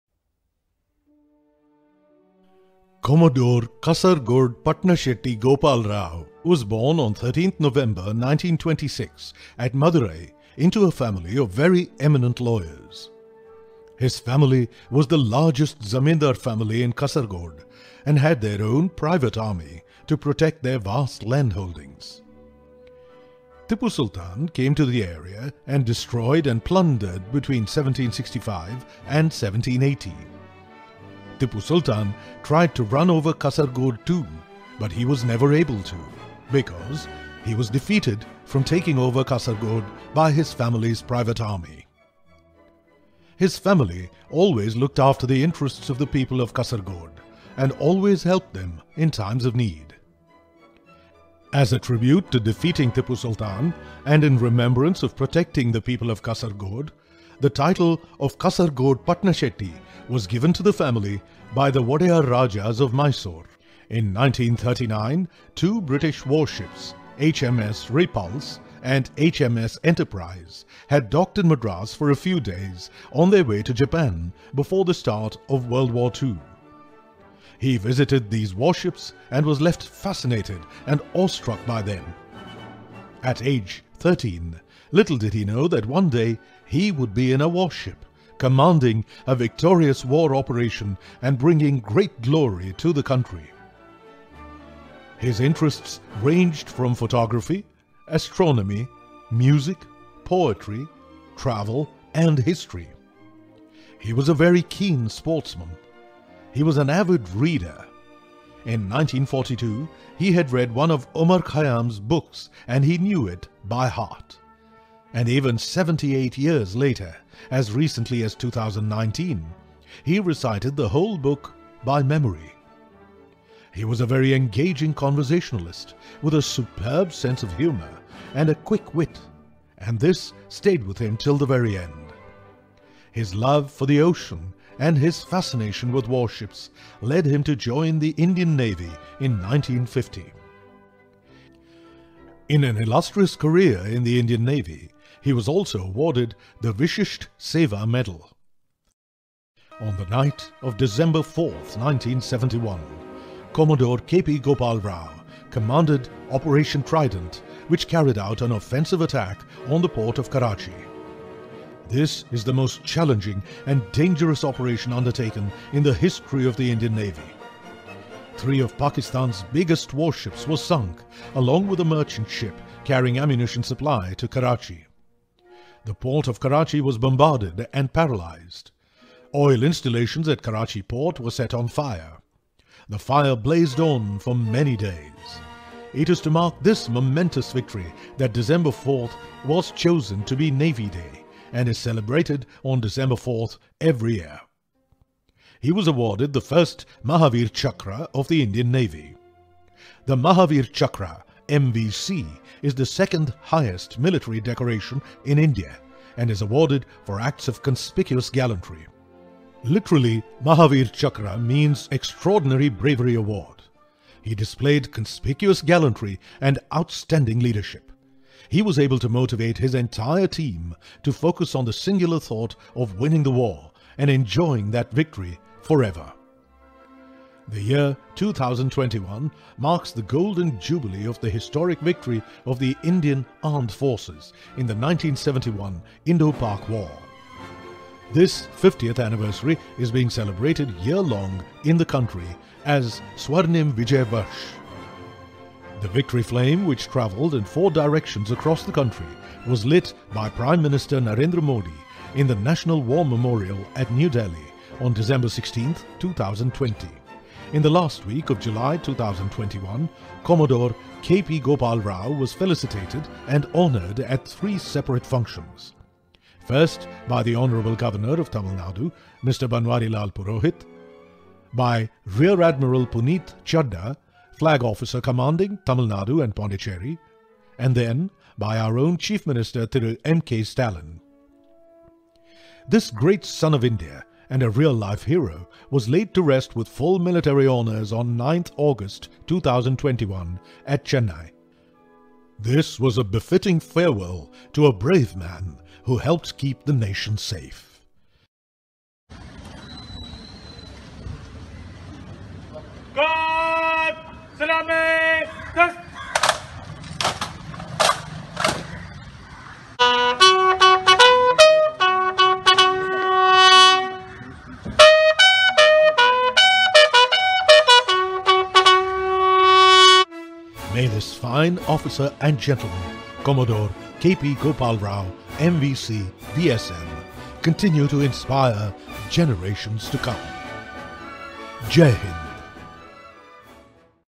Commodore K.P. Gopal Rao, MVC, VSM, Nov 13th 1926 - Aug 9th 2021
Naval War Hero of the 1971 Indo-Pak War and Commander of Operation Trident. Recipient of the first Mahavir Chakra of the Indian Navy.
December 4th was chosen as Navy Day to mark the success of Operation Trident. A Proud Son of India.

Voice-Over: Nilu
Editing: Vishnu Kumaraswami